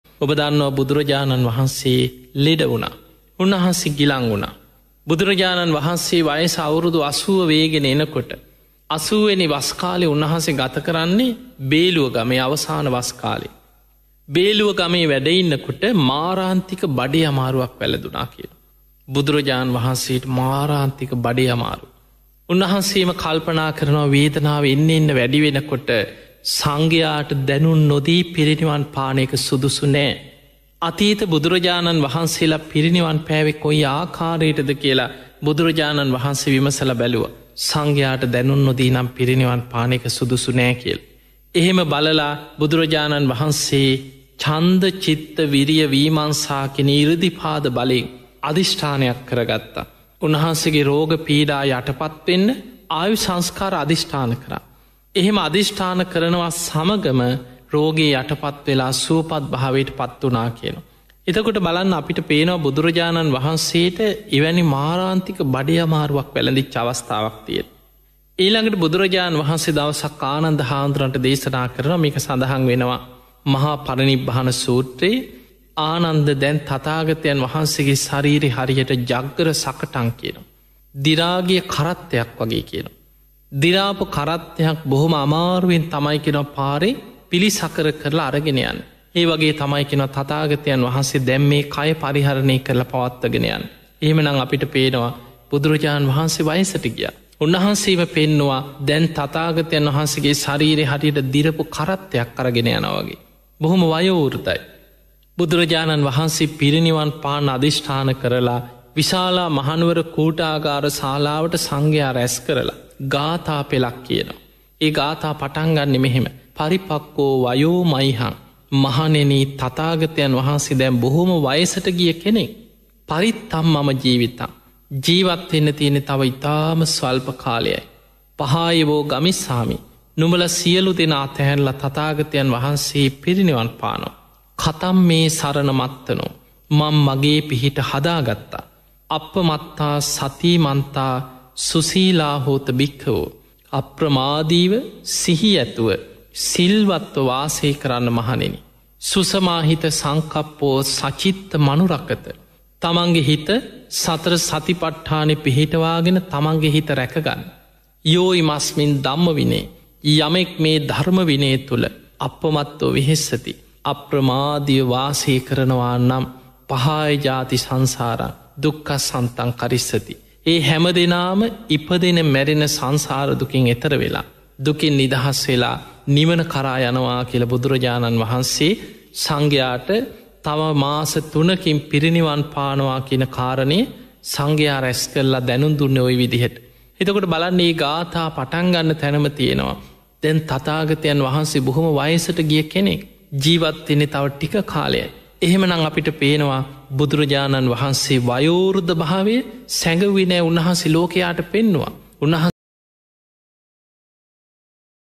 බුදු රජාණන් වහන්සේගේ අවසාන වස් කාලය.- Ven. Galigamuwe Gnanadeepa Thero
ගුවන් විදුලියෙන් ප්‍රචාරය වූ මෙම ධර්ම දේශනාව ධර්ම දානයක් ලෙස ඔබ වෙත පූජා කරමි. මෙය සියළු සත්වයාටම නිර්වානාවබෝදය පිනිසම හේතු වේවා! මෙම උතුම් ධර්ම දේශනාව කරනුයේ ඉතා සද් භාවයෙන් එම. පව් කිරිමෙන් තොර විම පිණිස. පව් කිරිමට බය විය යුතුයි..